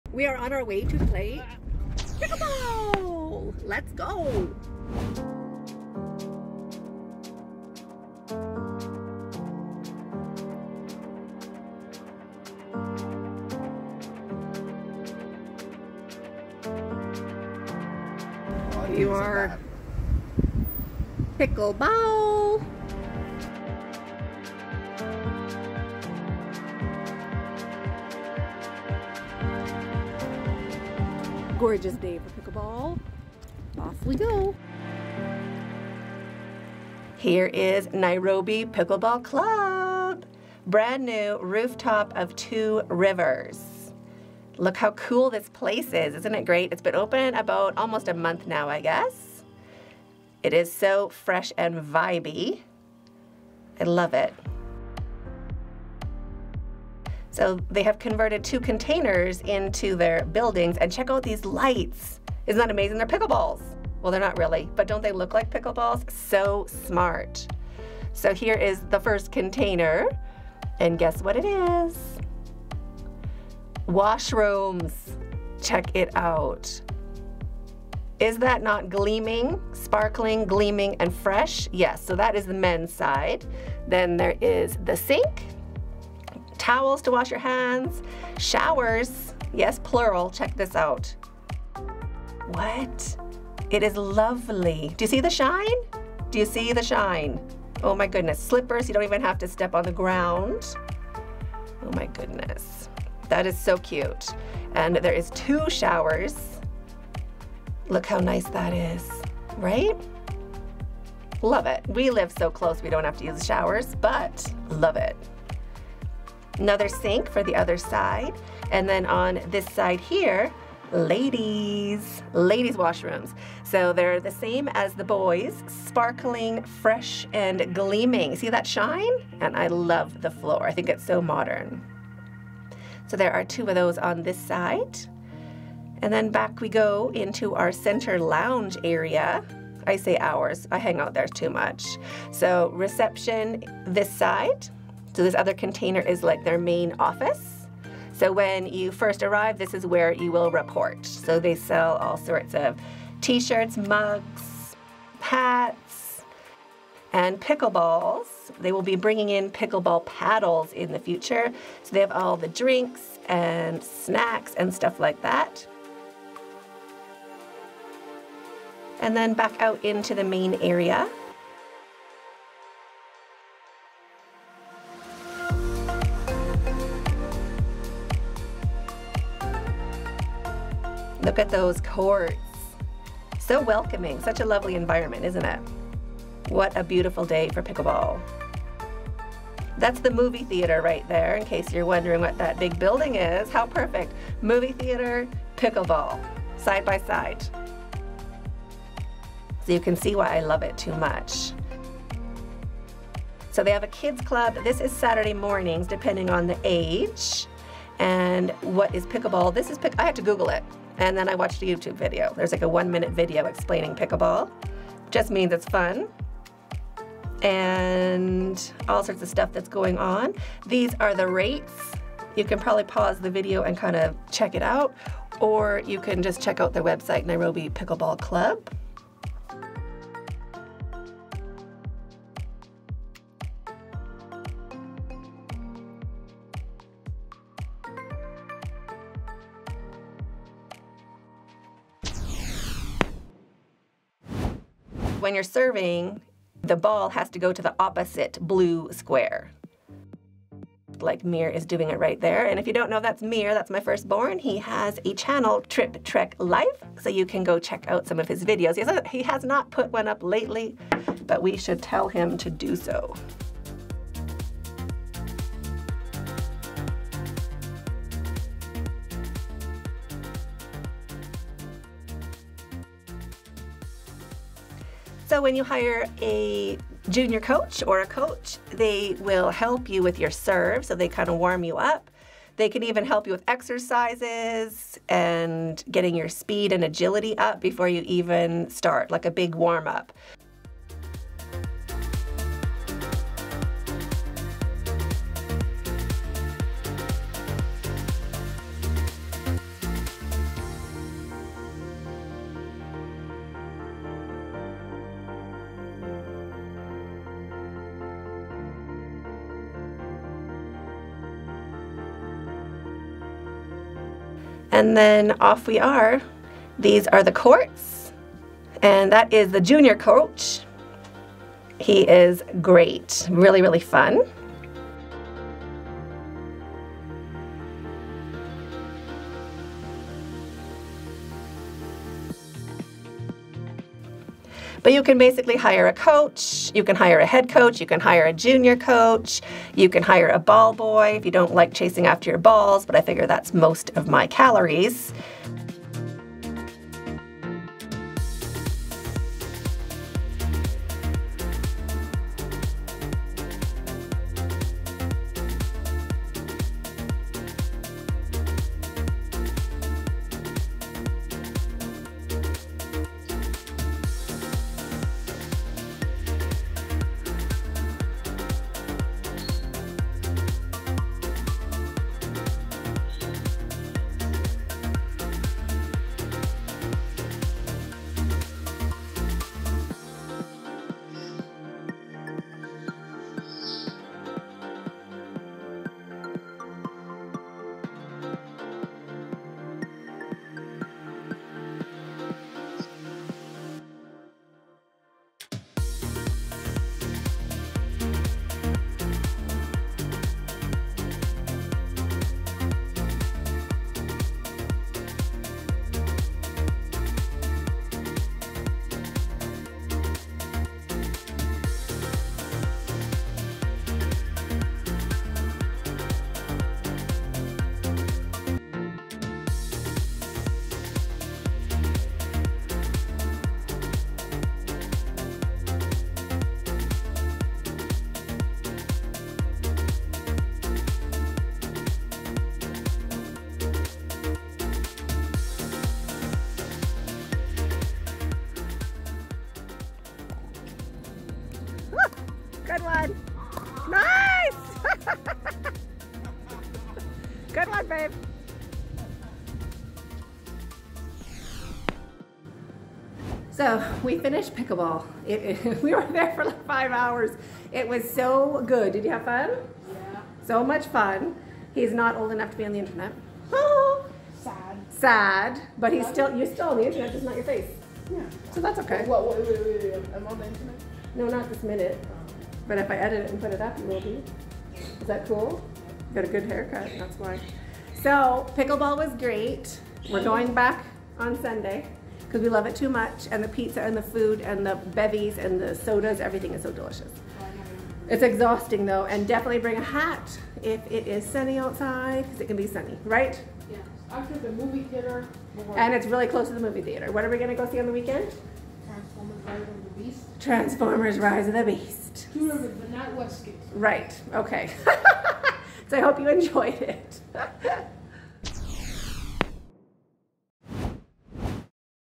Pickleball in Kenya? Fun Things to Do in Nairobi for the Whole Family
Check out this amazing NEW Nairobi Pickleball Club at Two Rivers!

🐠 Shopping on Amazon?

🐱 Shopping for Yarn?